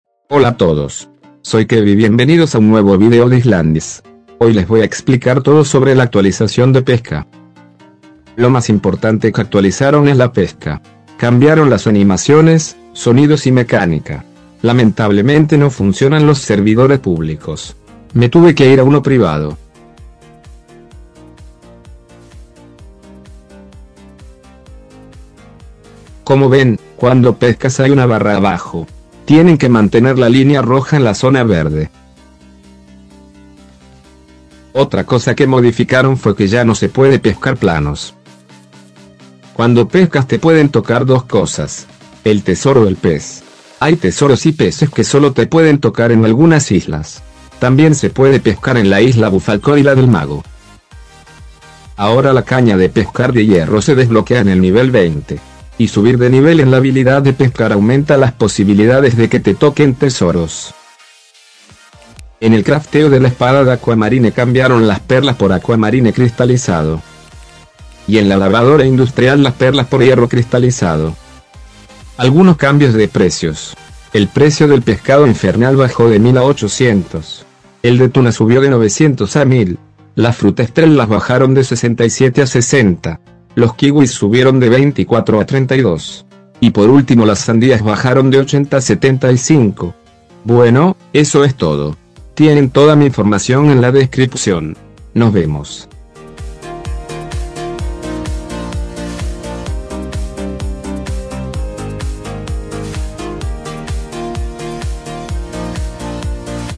*Nueva Actualización* Pesca🎣, tesoros🔎 y mucho más!
En este video les voy explicar TODO SOBRE LA NUEVA ACTUALIZACIÓN sobre las modificaciones
que le hicieron a la PESCA